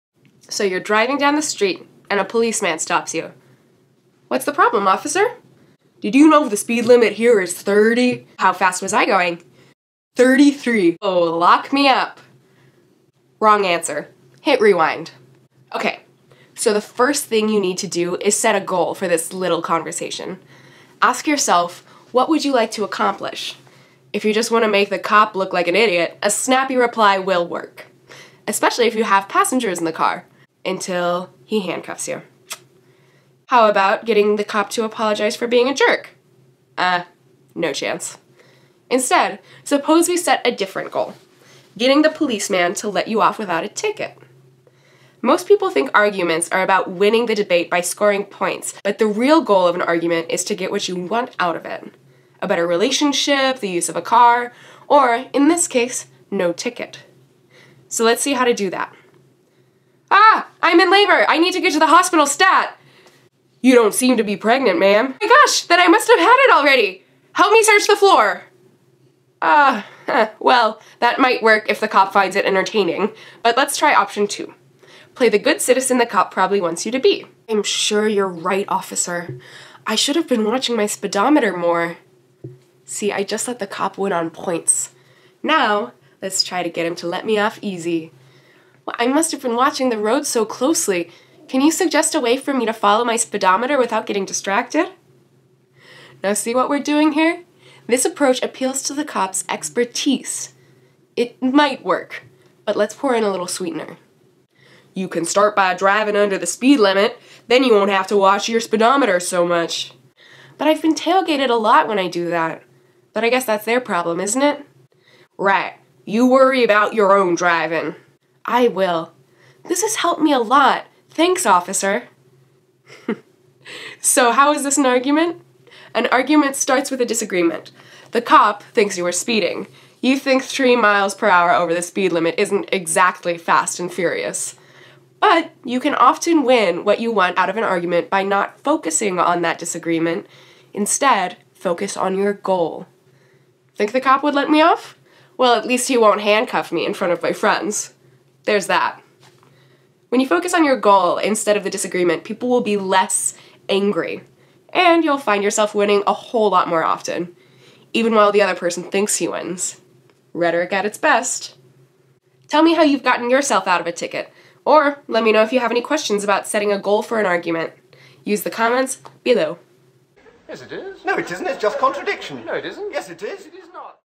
Get Out of a Ticket
The rhetorical tools you need to get a cop to let you off.